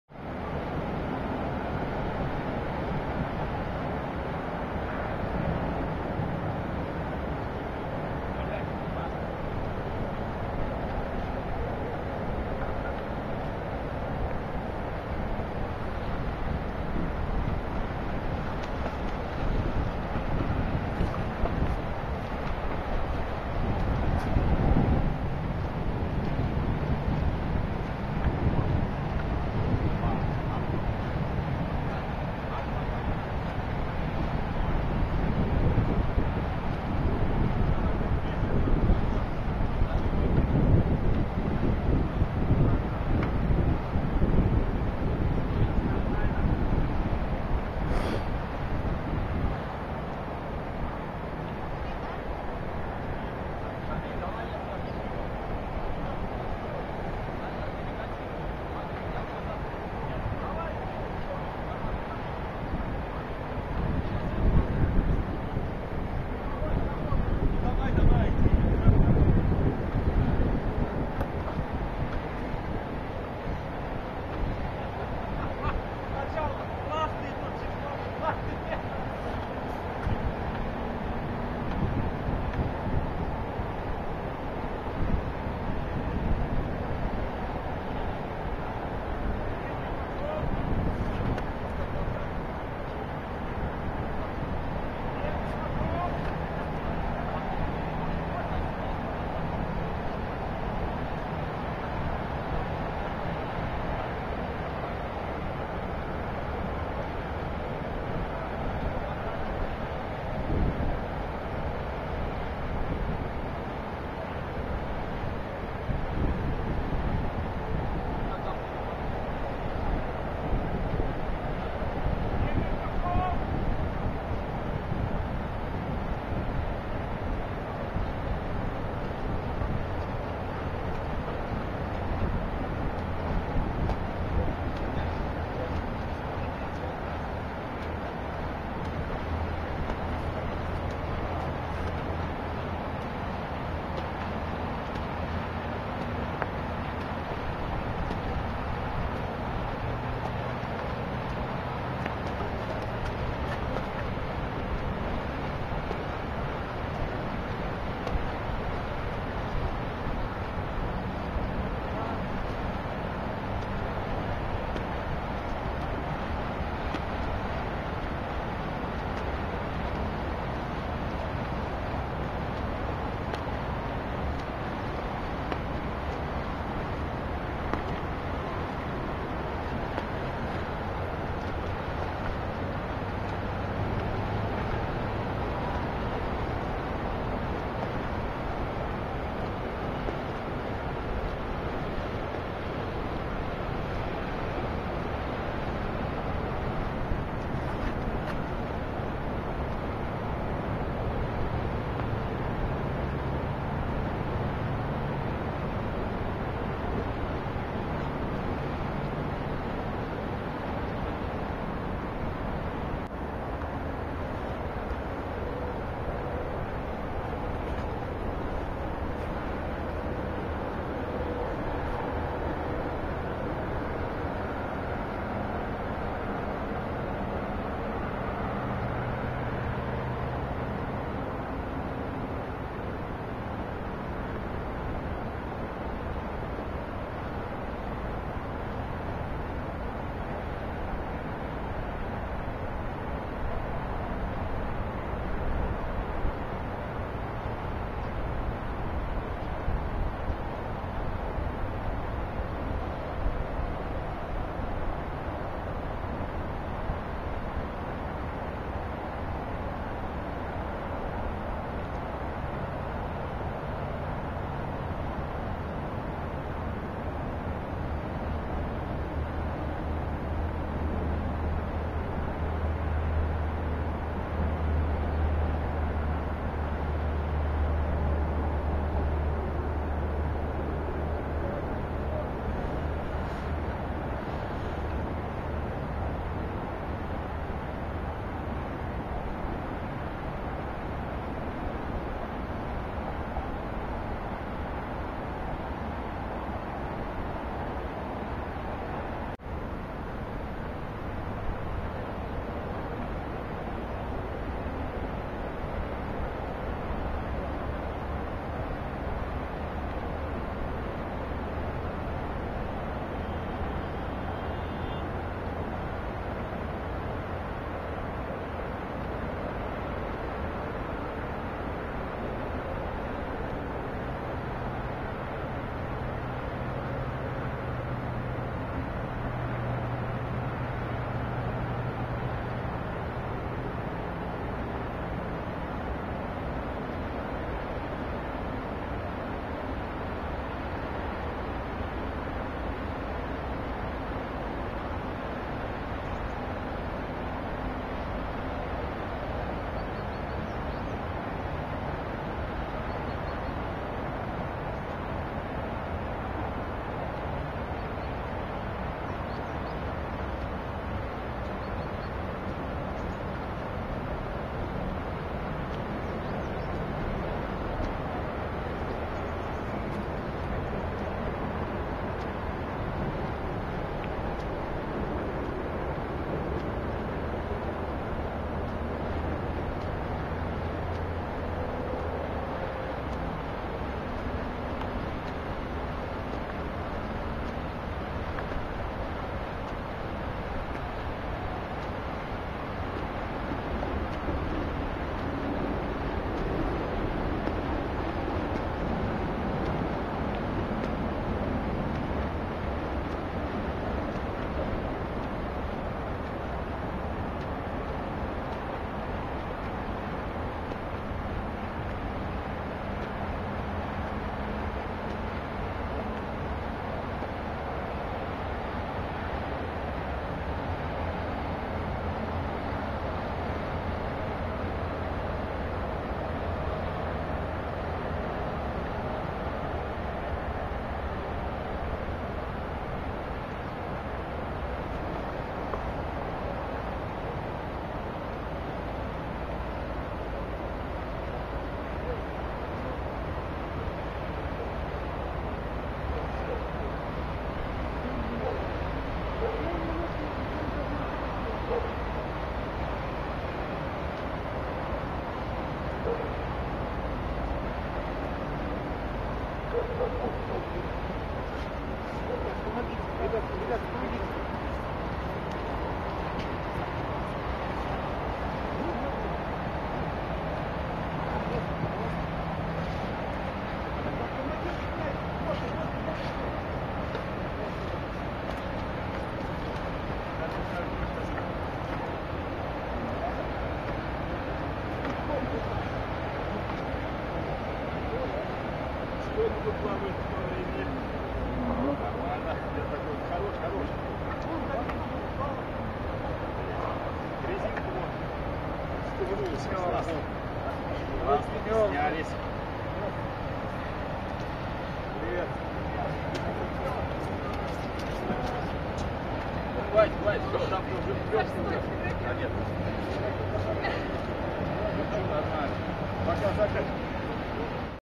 тренировка
подготовка к соревнованиям по зимнему плаванию